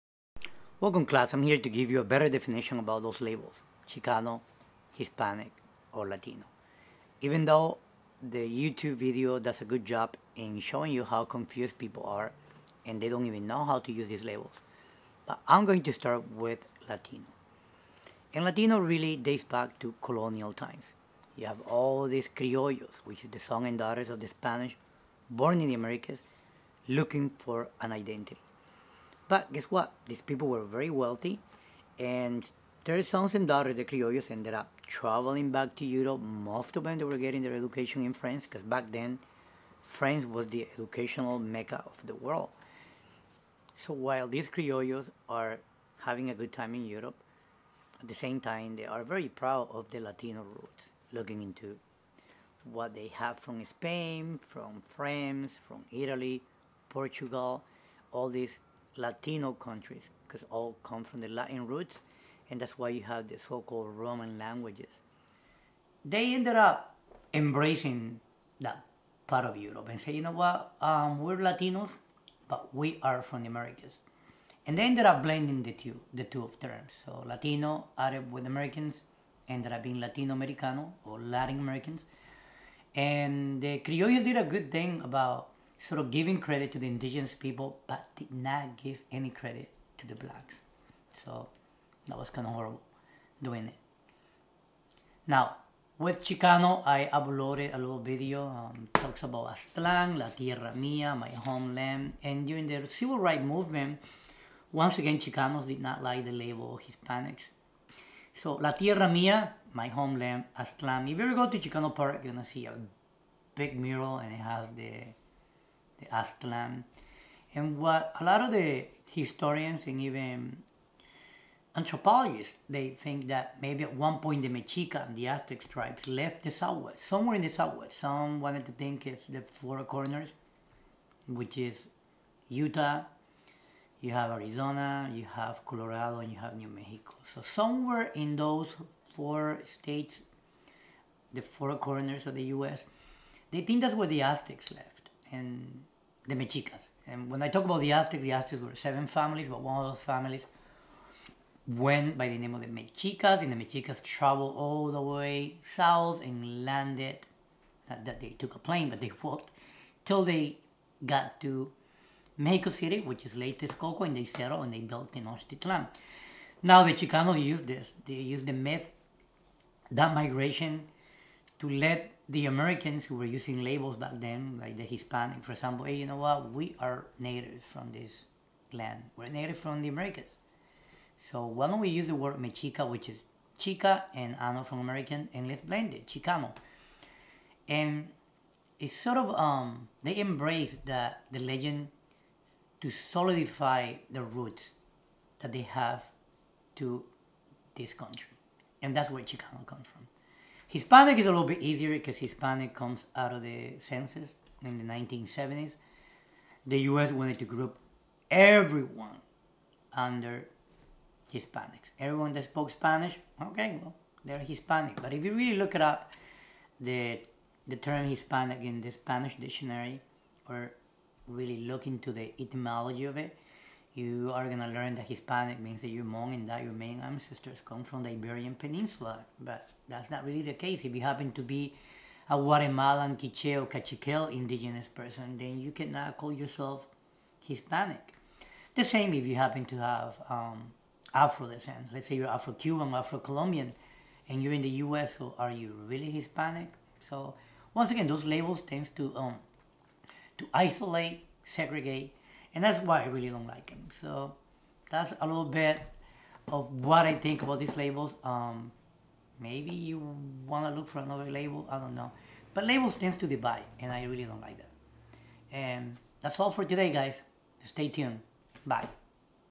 Confusing Labels: Latino-Hispanic-Xicano
What Each Means, How they Overlap, and What Sets them Apart. Hispanic and Latino are often used interchangeably though they actually mean two different things.